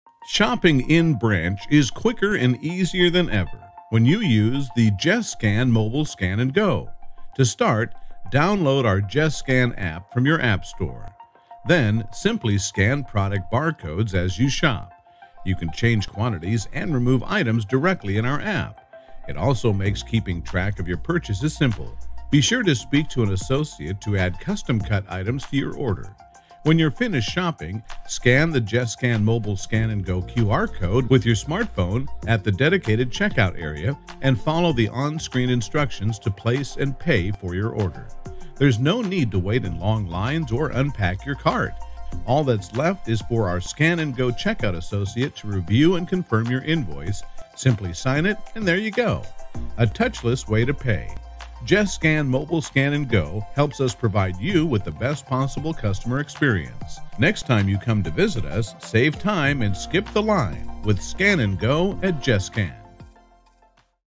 How To Use Scan & Go
Shopping in-store with Gescan is even quicker and easier when you use mobile Scan & Go to self-checkout directly from the Gescan App on your smartphone. Save time, skip the line! and download the Gescan App today to quickly be on your way.